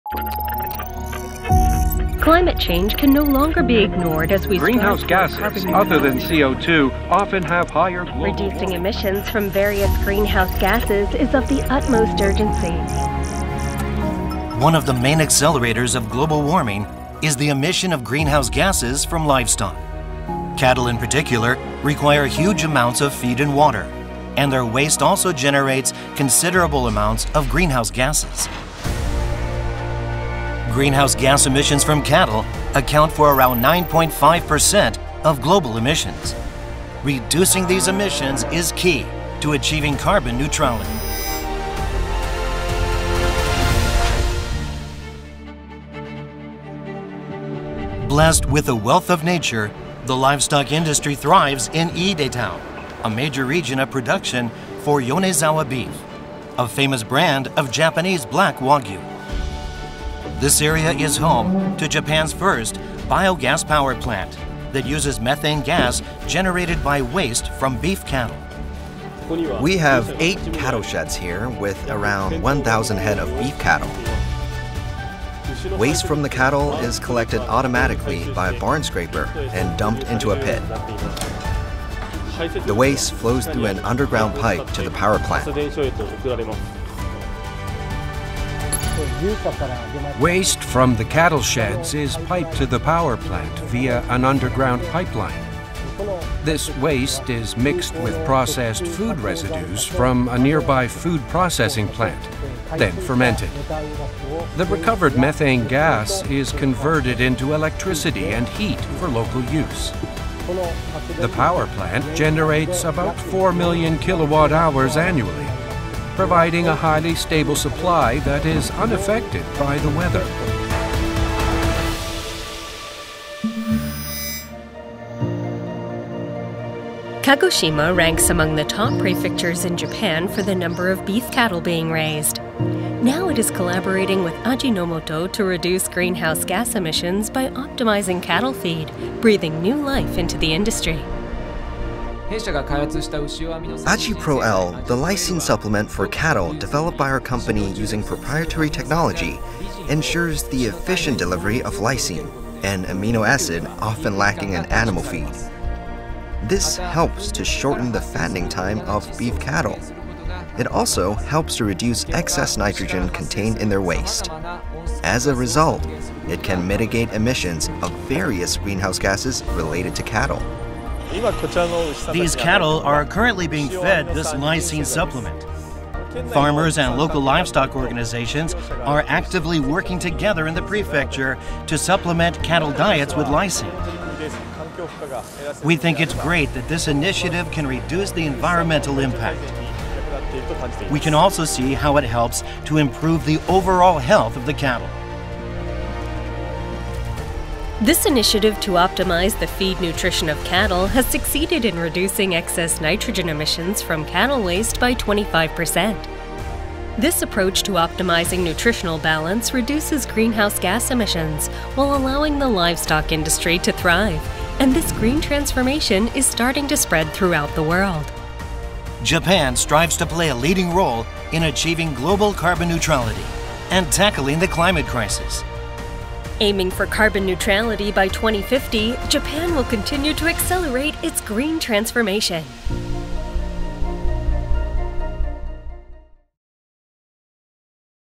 Japan’s Unique Approach to Cattle Emissions
Japan is combatting climate change by reducing greenhouse gas emissions from livestock. By converting methane from cattle waste into electricity, and optimizing cattle feed to reduce emissions, Japan strives to lead global efforts towards carbon neutrality and environmental sustainability.